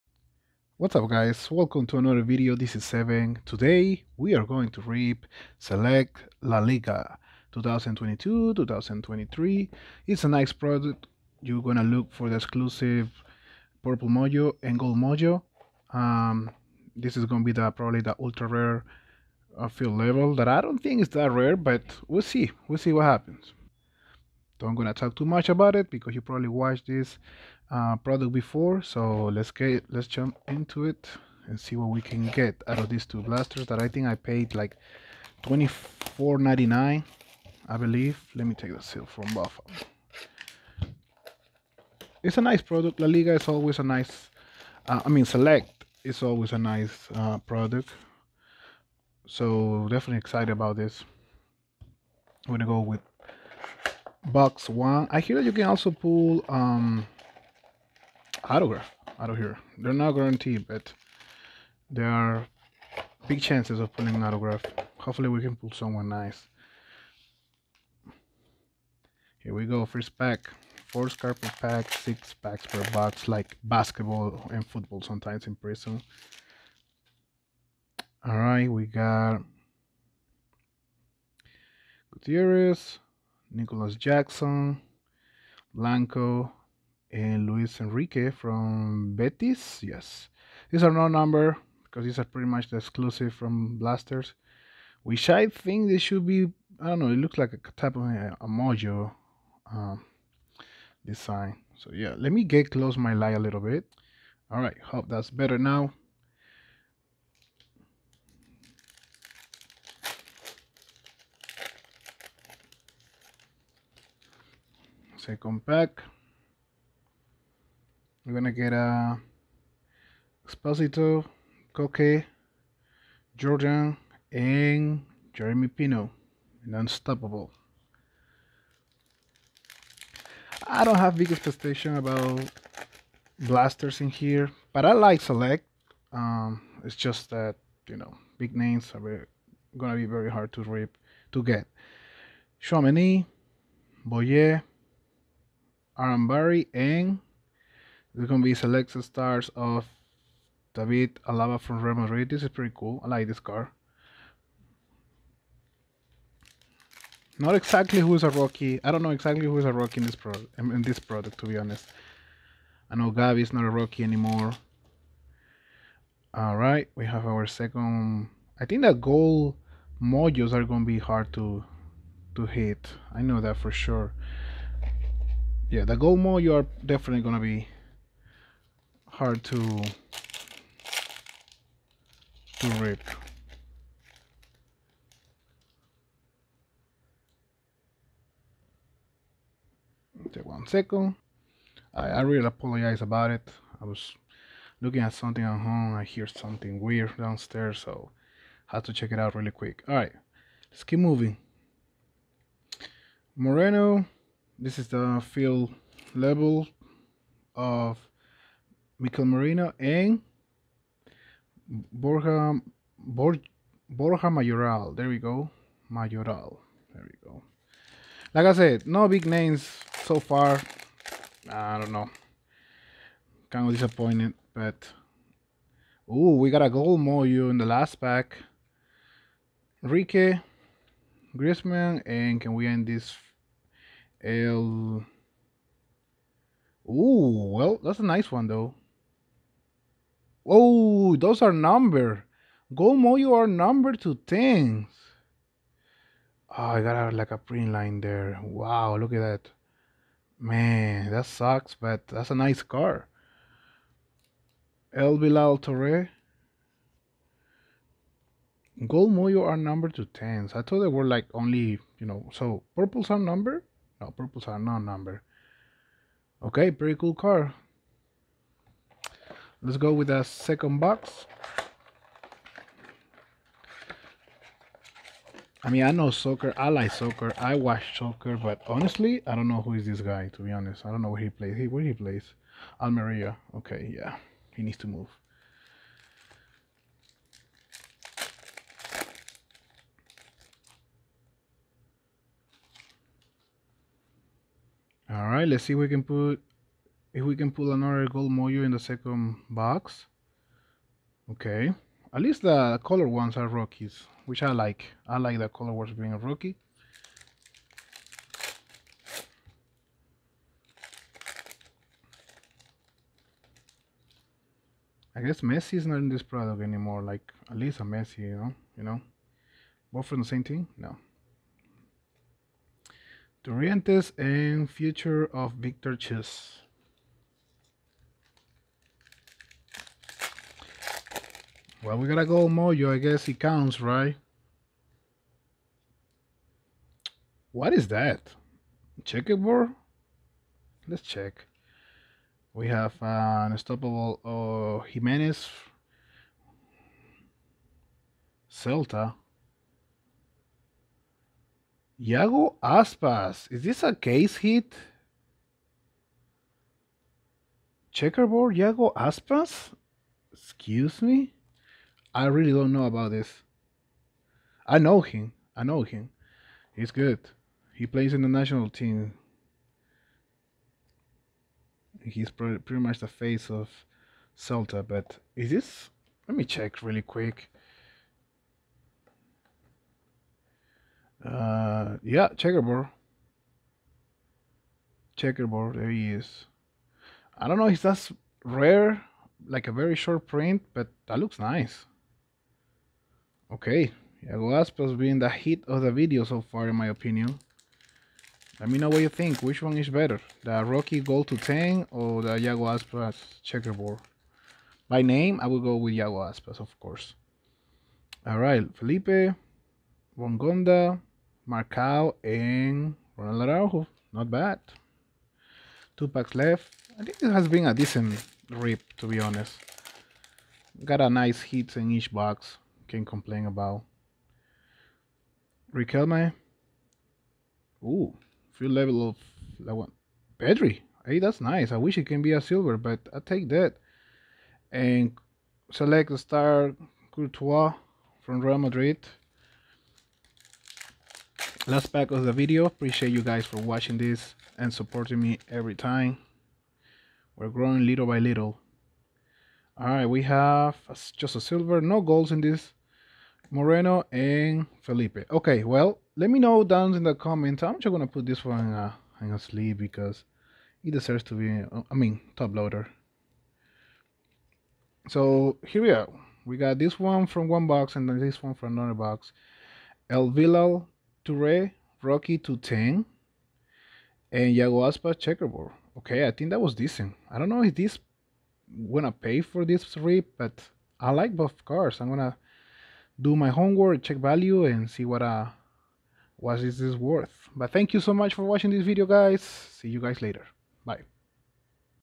2022-23 Select La liga - Retail Blasters Rookie Gold Mojo Pulled ! 🔥
2022-23 Select La Liga Retail Product

I pulled a Gold Mojo !